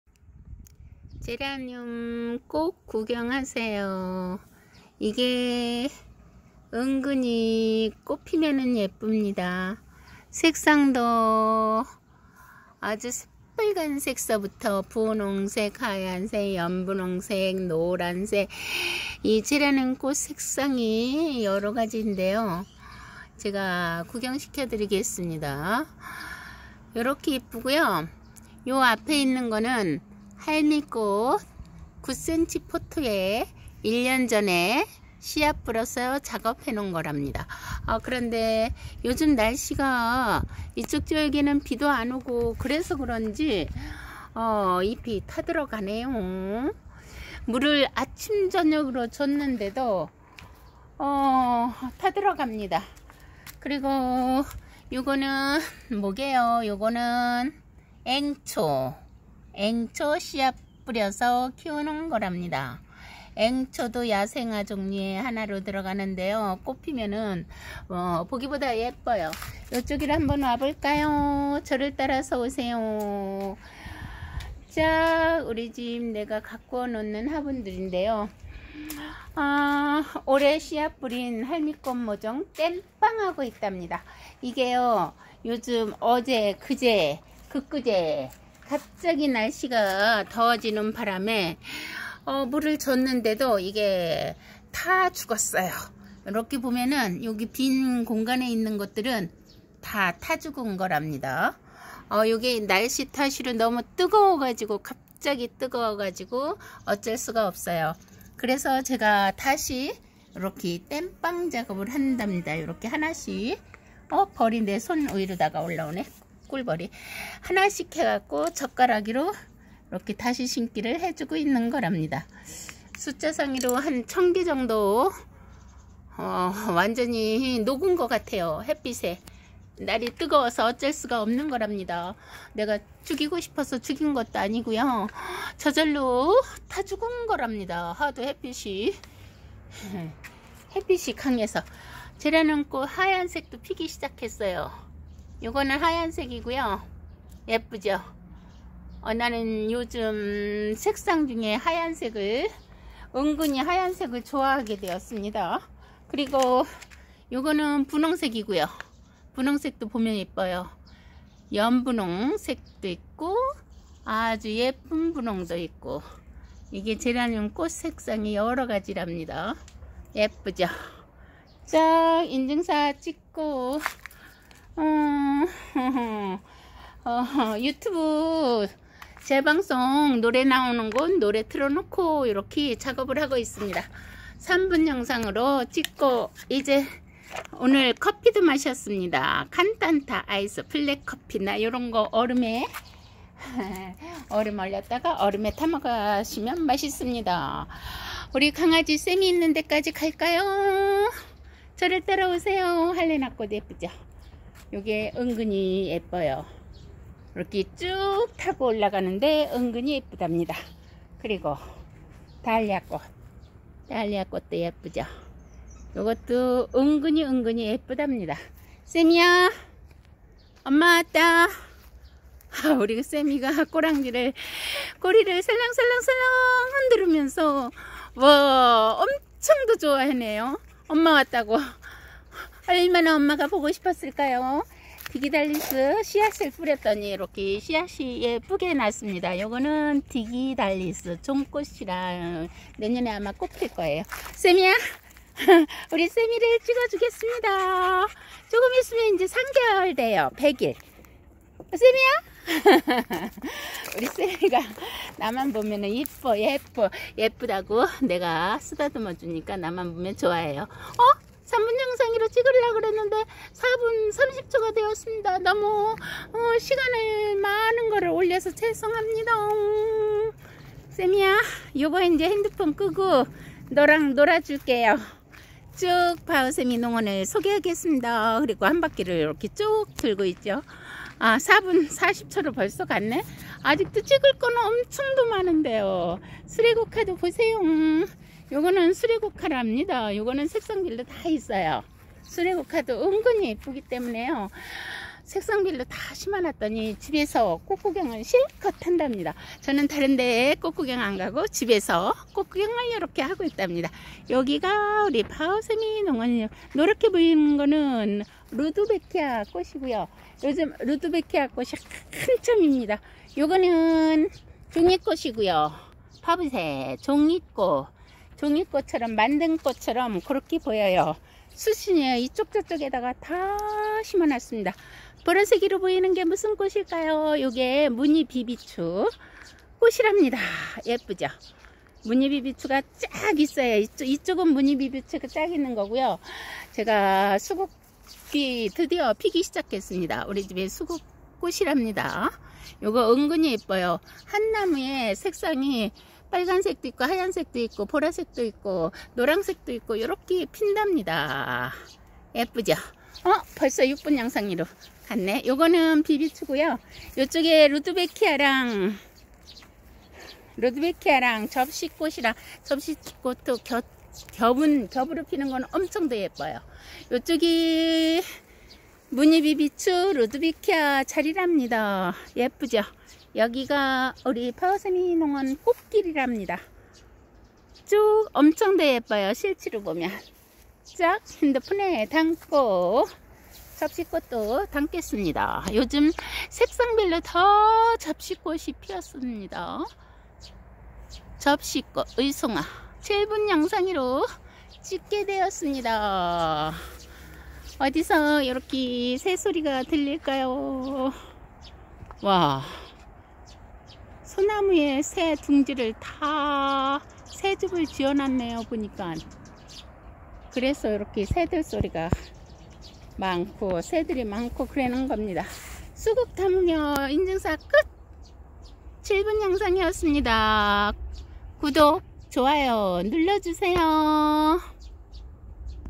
유친수!땜빵하기(할미꽃모종이식1차 땜빵)
날씨가 너무 더워서
50구 트레이에 심어 놓은
모종 땜빵하기~노래들으면서
땜빵하니 힘도 안들고
재미나다!